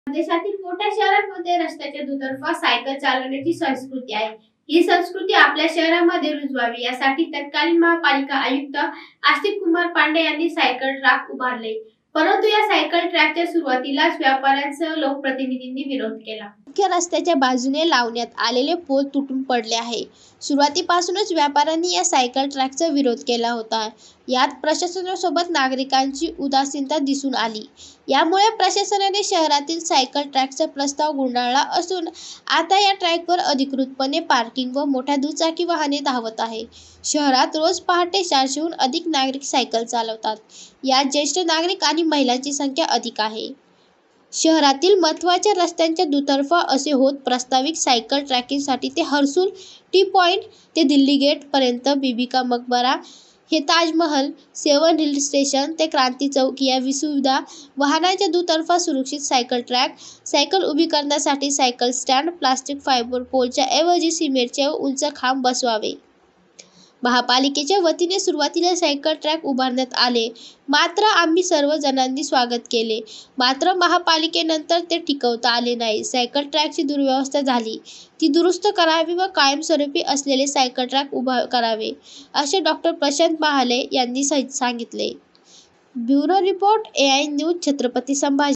वर्षभरातच सायकल ट्रॅक ‘पंक्चर’; चार कोटींचा प्रस्ताव गुंडाळला
परदेशातील मोठ्या शहरांमध्ये रस्त्याच्या दुतर्फा सायकल चालवण्याची संस्कृती आहे. ही संस्कृती आपल्या शहरामध्ये रुजावी यासाठी तत्कालीन महापालिका आयुक्त आस्तिक कुमार पाण्डेय यांनी सायकल ट्रॅक उभारले. परंतु, या सायकल ट्रॅकच्या सुरुवातीलाच व्यापाऱ्यांसह लोकप्रतिनिधींनी विरोध केला.
यातच प्रशासनाने केलेली धरसोड व सायकल ट्रॅककडे केलेले दुर्लक्ष आणि सायकल प्रेमींकडून प्रशासनावर दबाव न टाकल्याने शहरातील सायकल संस्कृतीचा प्रयोग फसला. वर्षभरात उभारण्यात आलेले सायकल ट्रॅकवर वाहने धावू लागल्याचे चित्र आहे.अगोदर नागरिक छंद म्हणून सायकल चालवायचे. यातून रेल्वे स्टेशन, क्रांती चौक, प्रोझोन मॉल ते गरवारे स्टेडियम या ठिकाणी सायकल ट्रॅक उभारण्यातही आले. मात्र, वर्षभरातच या ट्रॅकची बिकट अवस्था झाली आहे.